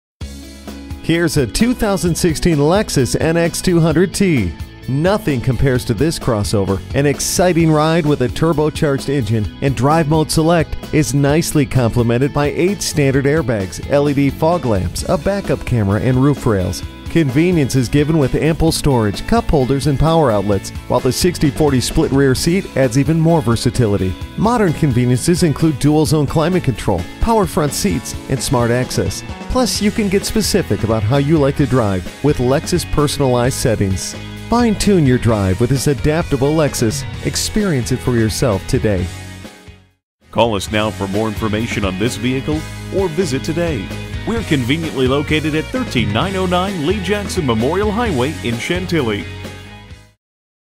New 2016 Lexus NX 200t Chantilly VA Washington-DC, MD NXG257604 - SOLD
Year: 2016
Make: Lexus
Model: NX 200t
Trim: AWD 4dr
Engine: L 4 Cyl.
Color: WHITE
Interior: RED
Stock #: NXG257604
VIN: JTJBARBZ5G2057604
Packages P Z2 Cargo Net/Cargo Mat/Whl Locks/Rr Bumper Protector Accessories F EH Electrochromic (Auto-Dimming) Inside Rear ViewMirror; and Lexus Homelink Garage Door Opener F FK 18 F SPORT Wheels w/Summer Tires (235/55R18) F NV Navigation Package Includes:Remote Touch Interface, Lexus Enform Remote,Lexus Enform Destination (includes 1-year trialsubscription); App Suite (complimentary),10-Speaker Lexus Premium Sound System F OQ Electrochromic (Auto-Dimming) Outer Mirrorswith Blind Spot Monitor (BSM), Rear Cross TrafficAlert, Reverse Tilt, Heated, Memory F PA Intuitive Parking Assist w/Clearance & Back Sonar F PB Power Back Door F PT Premium F SPORT PackageHeated Front Seats, Power Tilt & Slide Moonroof,Memory: Power Tilt/Telescopic Steering Column,Power 10-Way Driver Seat including Lumbar Support F WE Heated Perforated Leather Trimmed Steering Wheelwith Paddle Shifters

Address:
13909 Lee Jackson Memorial Hwy